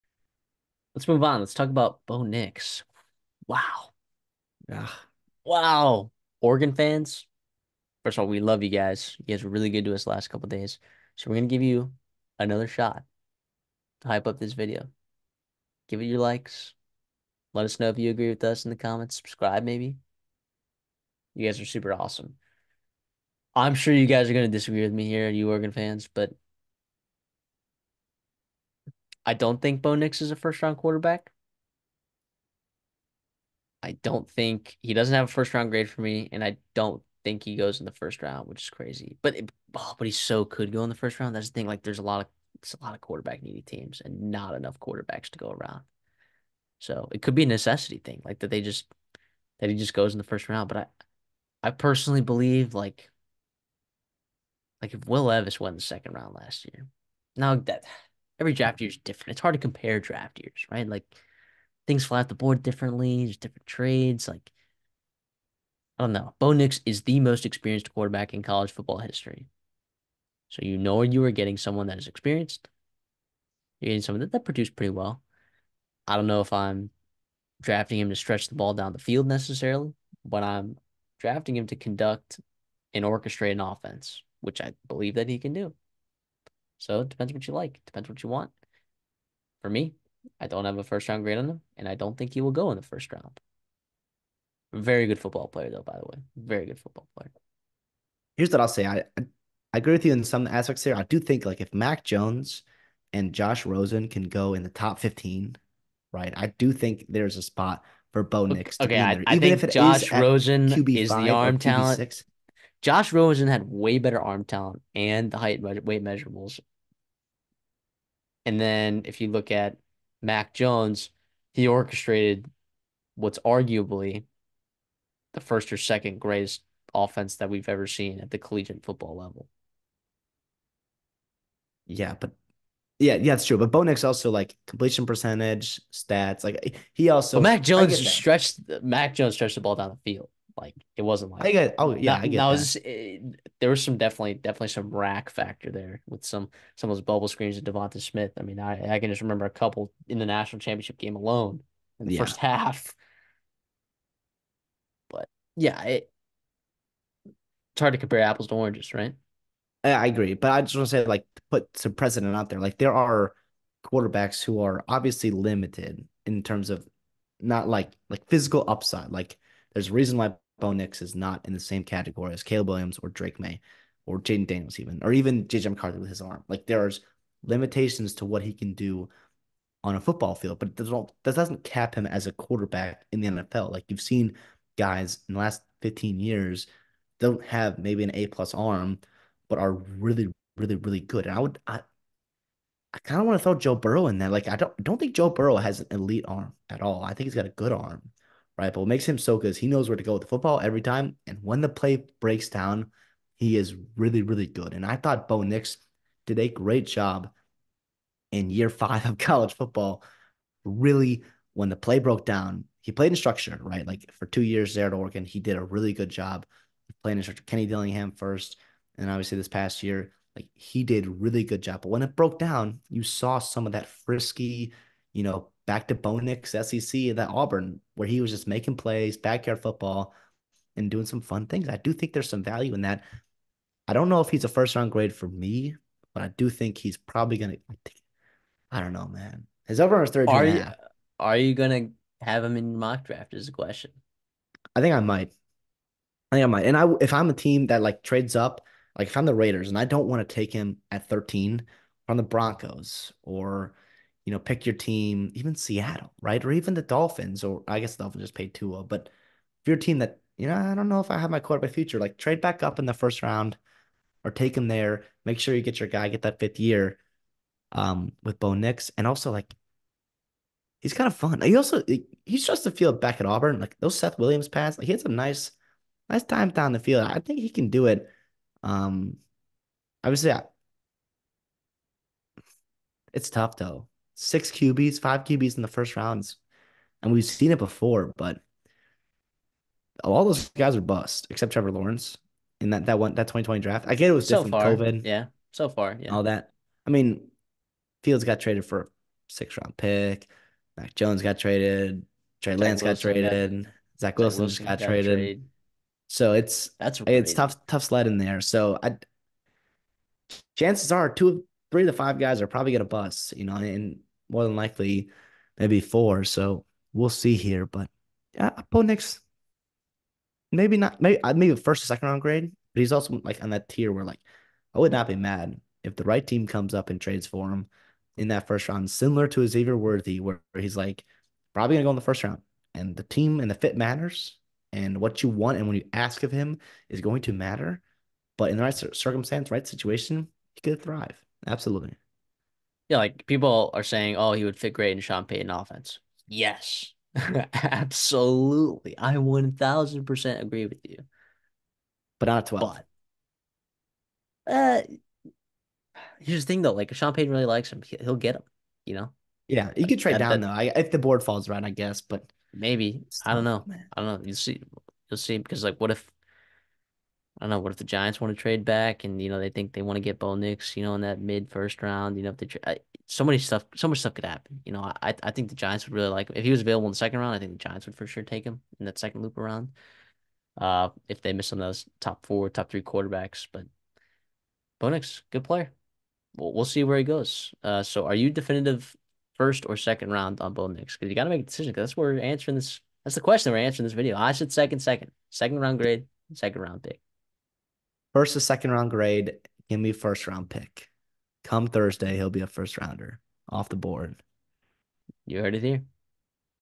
1st or 2nd Round? | Bo Nix 2024 NFL Draft
Oregon's Bo Nix is a great player... but should he go in the first or second round?

Note: We do not own the highlights featured in this video.
Credit to @nbcsboston